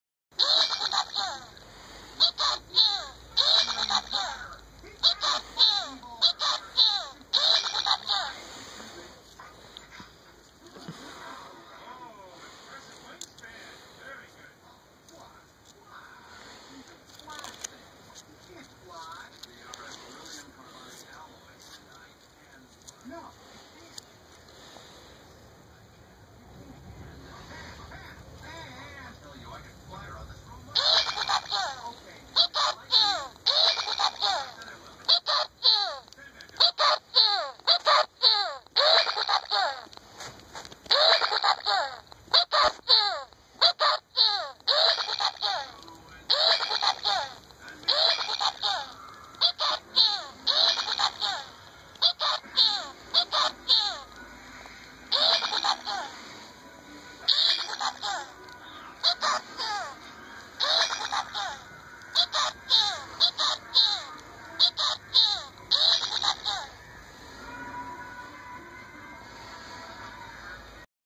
Pikachu keychain
Its one of my old Pikachu keychain toys that I had when I was younger. When you squeeze its stomach it makes noise. :)